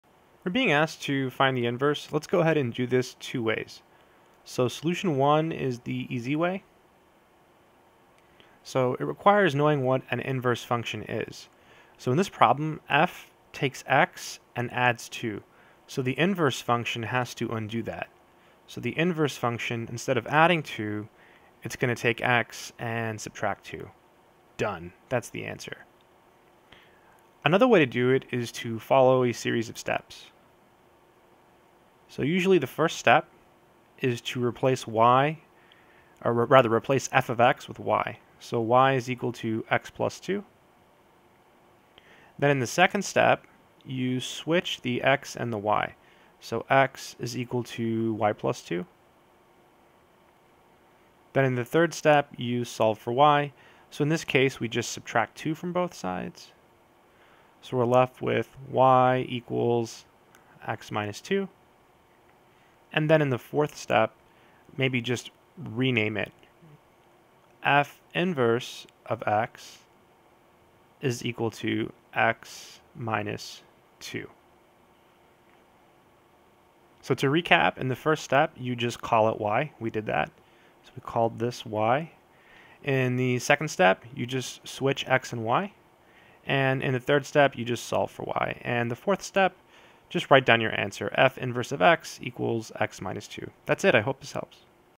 Finding the Inverse of the Linear Function f(x) = x + 2
Finding the Inverse of the Linear Function f(x) = x + 2. Two solutions are given.